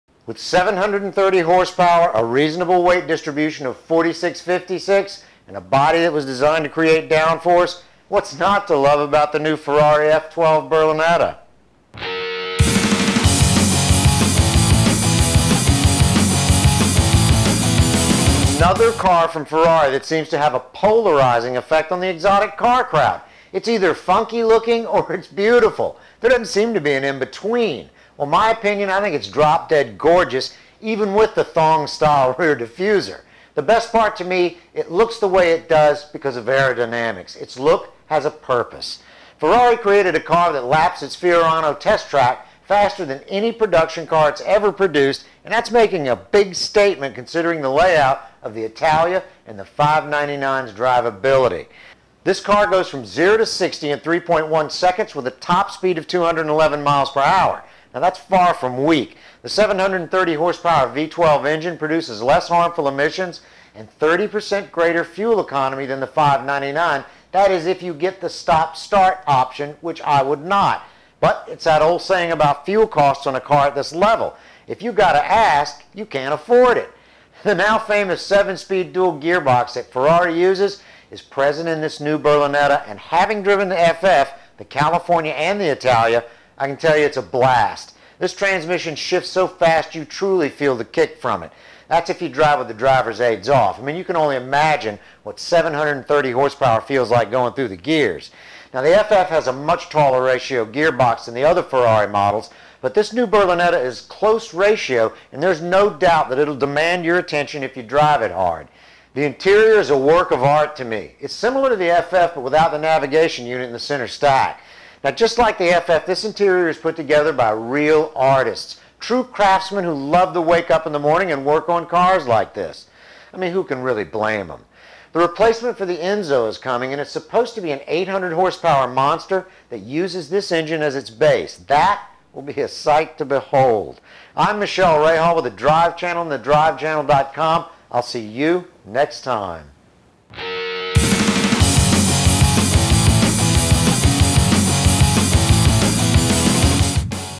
New Ferrari F12 Berlinetta Unveiled at Geneva Auto Show
Ferrari's new replacement for the 599 GTB is the F12 Berlinetta. It unveiled at Geneva, but Ferrari had leaked the details right at show start. This 720 Horsepower, front/mid-eangine V12 is the most powerful V12 developed by Ferrari in a production car.